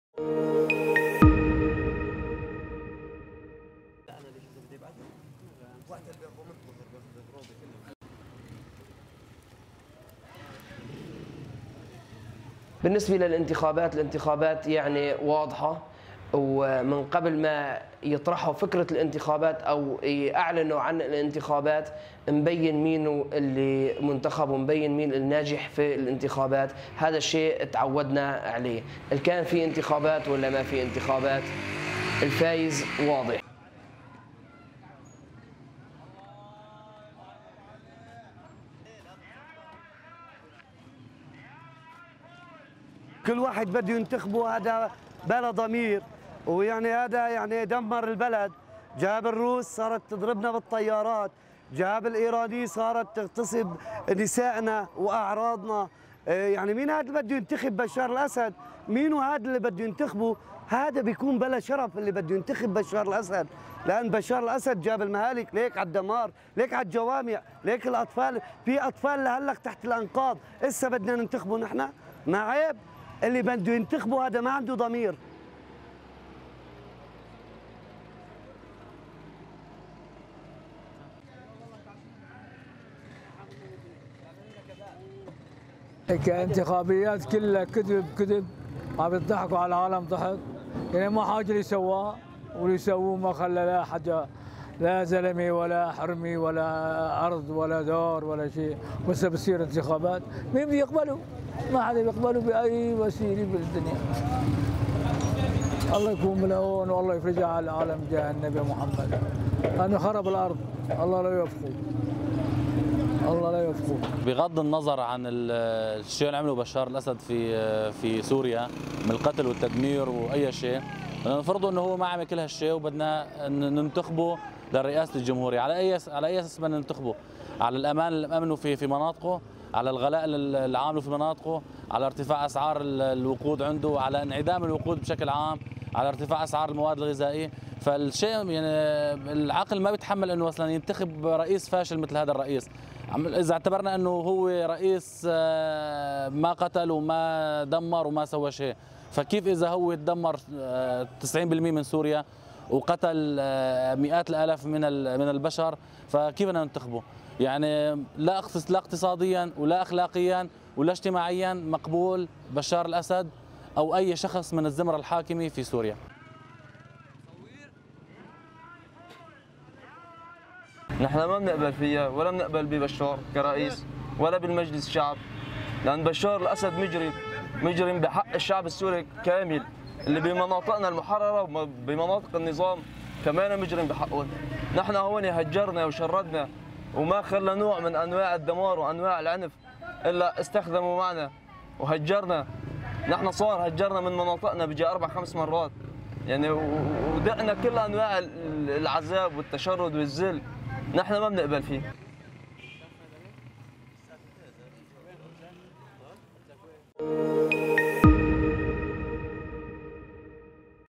Syria – Syrian opposition areas to oppose re-electing Bashar al-Assad as president
The citizens of Jabal al-Zawiya in Idlib countryside, northern Syria, have condemned the People's Assembly’s declaration of the opening of the door for candidacy for the presidential elections in Syria, scheduled for mid-May. They refused to re-elect the current Syrian President Bashar al-Assad, because of what he has done against them in terms of killing and displacing civilians.